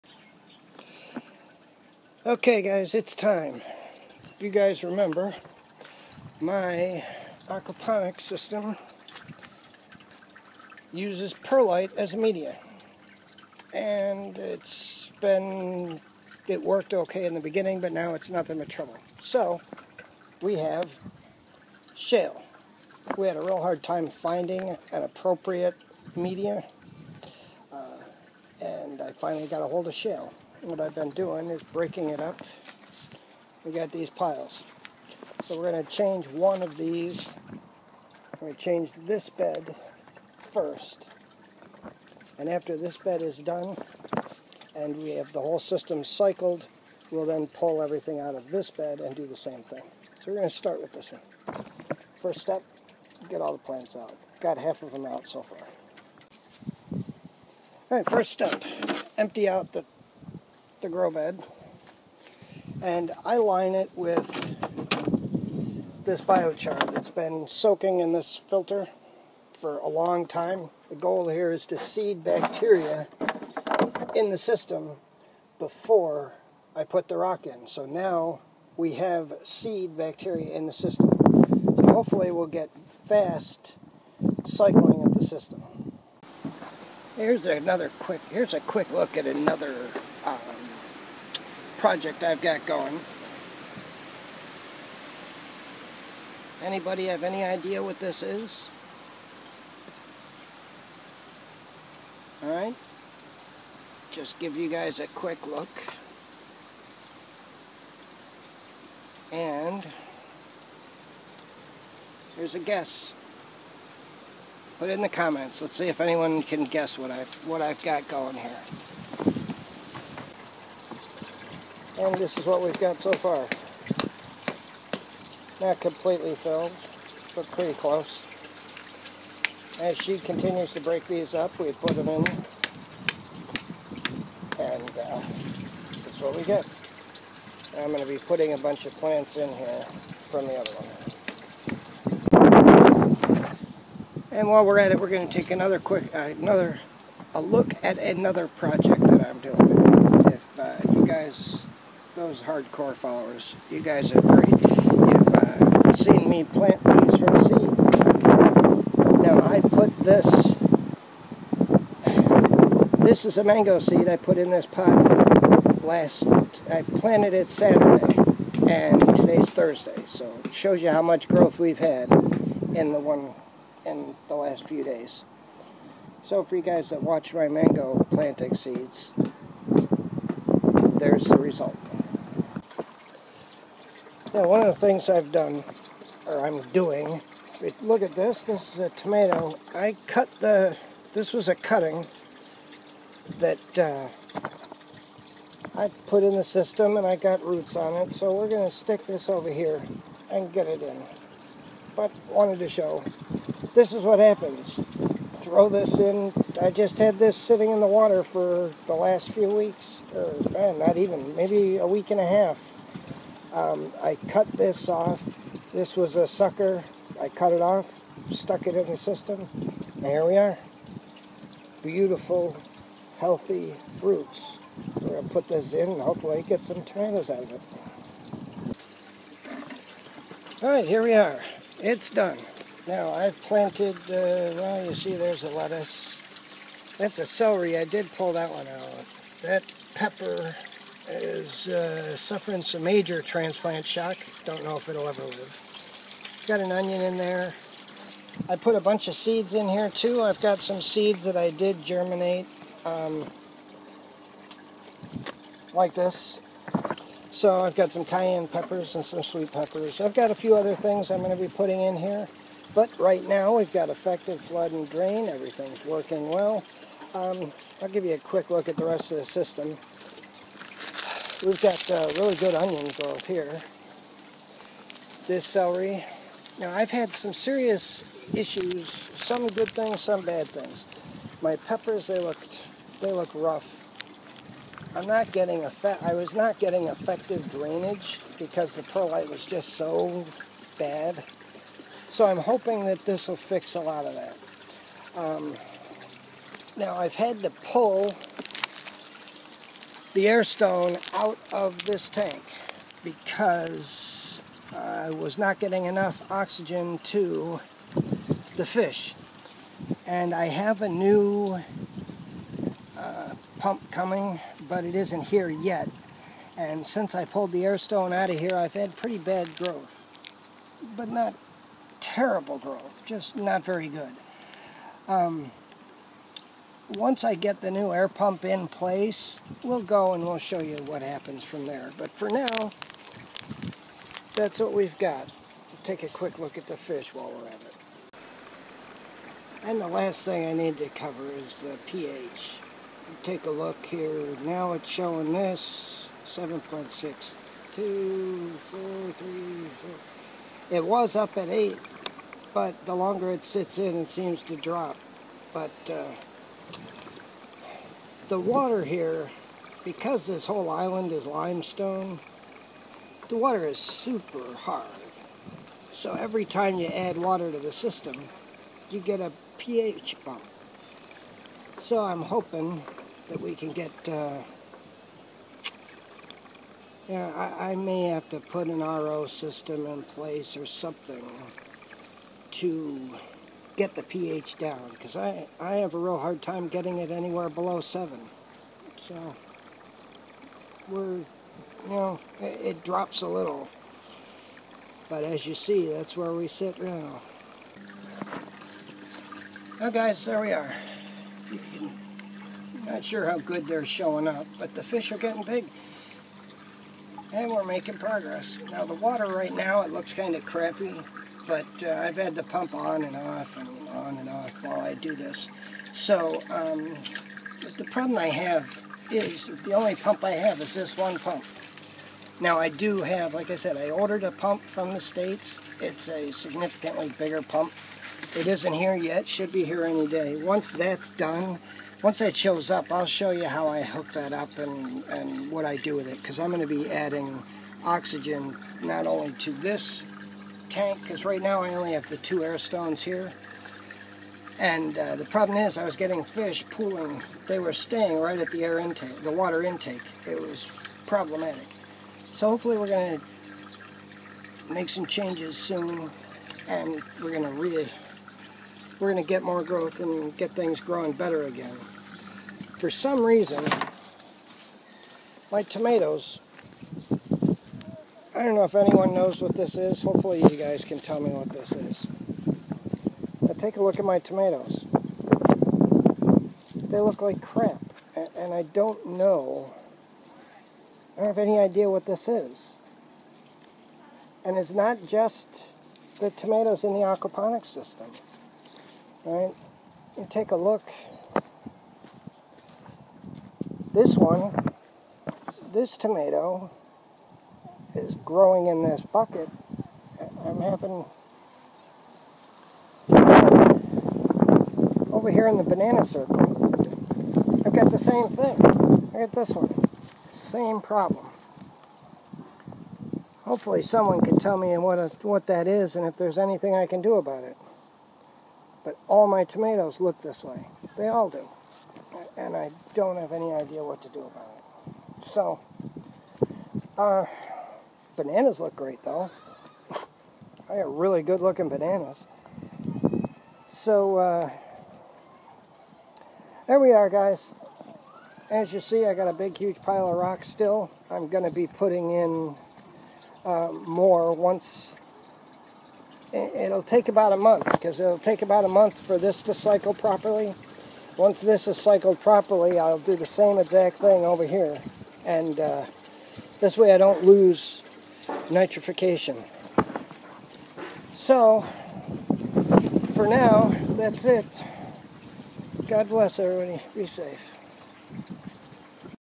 Aquaponics update: new media going in
Finally new media for the flood and drain beds.
Mystery project taking guesses.